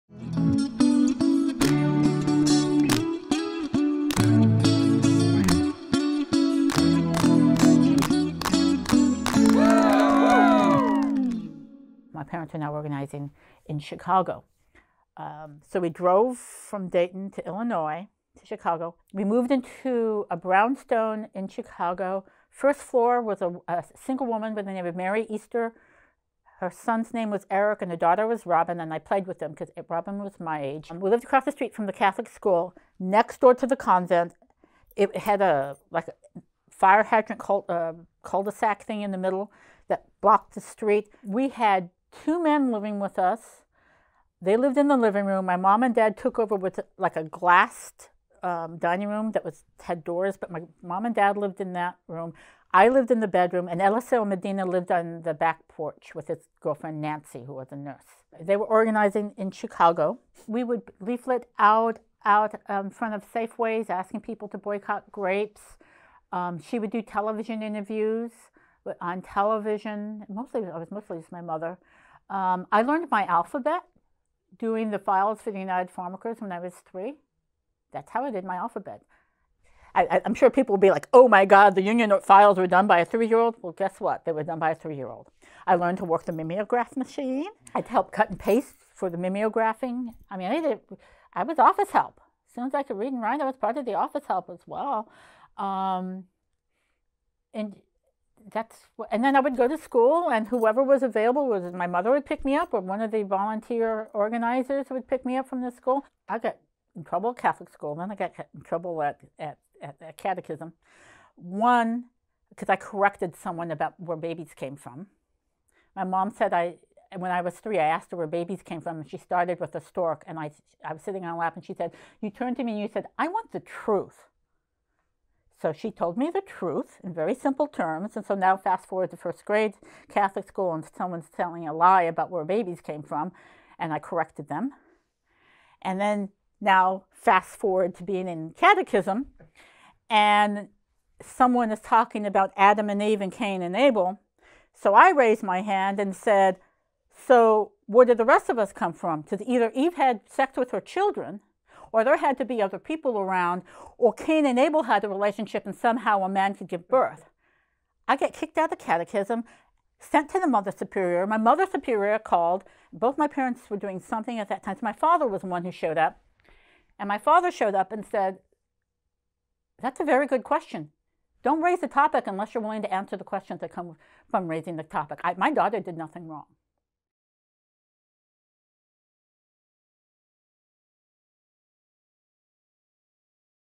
Carmen Ramos Chandler On Her Family Organizing in Chicago
Carmen Ramos Chandler talks about her family moving to Chicago to help organize the grape boycott and about her memories as a child of two organizers. Marta Valier, José Luis Benavides, and Brandon Lien conducted this oral history interview. Brandon Lien edited this video.